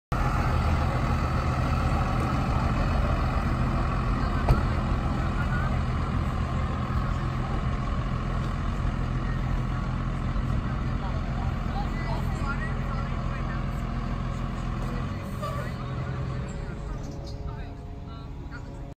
Bus 260: 2003 Blue Bird TC2000 FE (Cummins ISB)
This was taken in June during the 2018-19 school year. This bus is now on its 16th year in service. Go go 260!